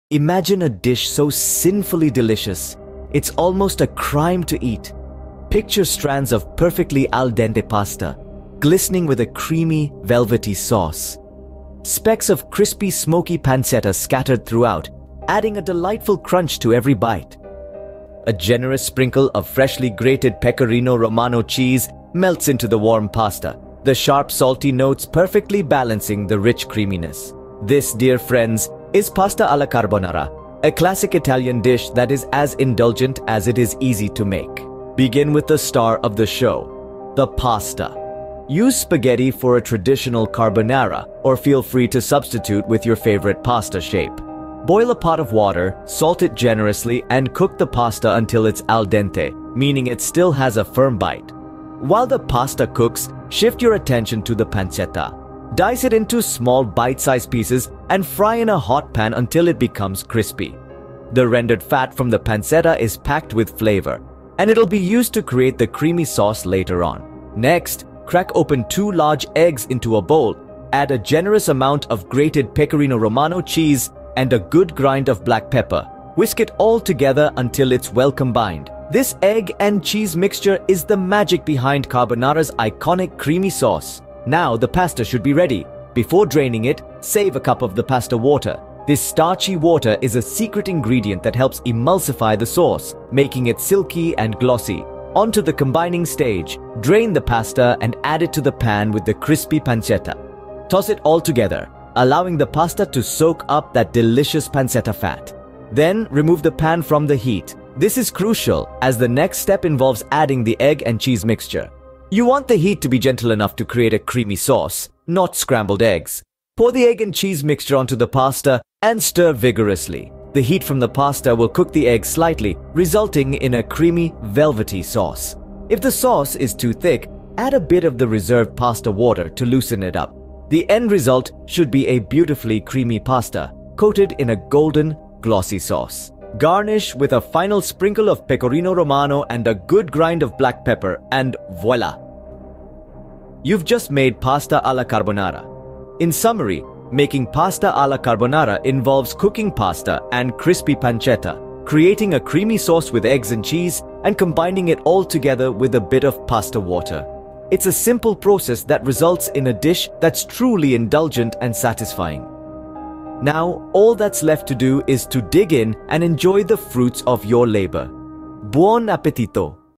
🍝 Mastering Pasta alla Carbonara
Welcome to the  Cookit4Me kitchen where we unravel the secrets of crafting the perfect Pasta alla Carbonara! 🍽️ Join us in this immersive cooking experience as we guide you through each step, ensuring your Carbonara game is on point.

🔍 What's Inside:

Authentic Italian recipe and technique
Pro tips for achieving the creamiest texture
Choosing the right ingredients for flavor perfection
Elevating your pasta skills to culinary mastery
👨‍🍳 Become a Carbonara Maestro:
Unleash your inner chef and impress your taste buds with the savory symphony of flavors. Don your apron, grab your ingredients, and let's elevate your pasta-making skills together!

🍝 Ingredients:

200g (7oz) of spaghetti
100g (about 1 cup) of guanciale or pancetta, diced
2 large eggs
1 cup (about 100g) of Pecorino Romano cheese, grated
Salt for pasta water

🔔 Ring that bell icon to stay updated on our latest culinary adventures. Your journey to becoming a kitchen maestro starts with a single click!